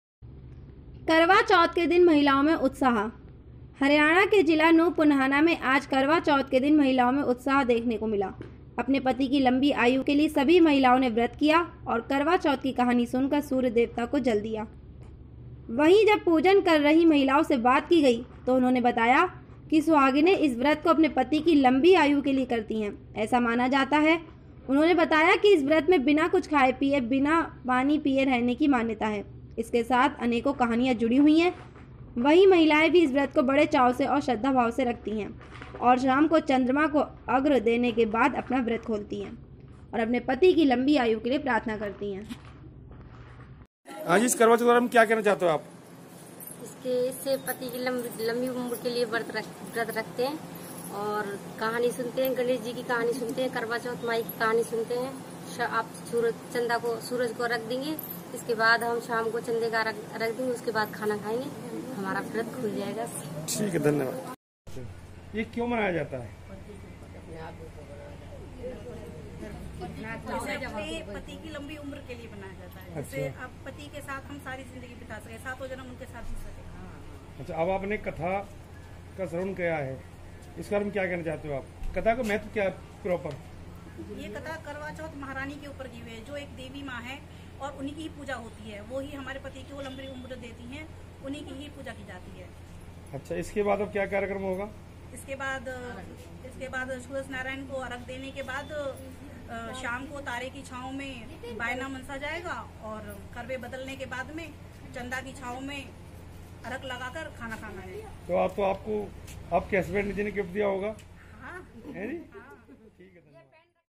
नूह पुन्हाना करवा चौथ के दिन महिलाओ में उत्साह
करवा चौथ के दिन महिलाओ में उत्साह
पुन्हाना

  हरियाणा के जिला नूह पुन्हाना में आज करवा चौथ के दिन महिलाओ में उत्साह देखने को मिला अपने पति की लंबी आयु के लिए सभी महिलाओं ने ब्रत किया और करवा चौथ की कहानी सुनकर सूर्य देवता को जल दिया
वीओ -  वहीँ जब पूजन कर रही महिलाओं से बात की गयी तो उन्होंने बताया की सुहागिने इस व्रत को अपने पति की लम्बी आयु के लिए करती हैं ऐसा माना जाता हैं ,,, उन्होंने बताया की इस व्रत में बिना कुछ खाये पीए,बिना पानी पीए रहने की मान्यता है ,,इसके साथ अनेको कहानियां जुडी हुई हैं ,,वहीँ महिलाएं भी इस व्रत को बड़े चाव से और श्रद्धा भाव से रखती हैं औरशाम को चन्द्रमा को अर्ध्य देने के बाद अपना व्रत खोलती हैं। और अपने पति की लम्बी आयु के लिये प्रार्थना करती हैं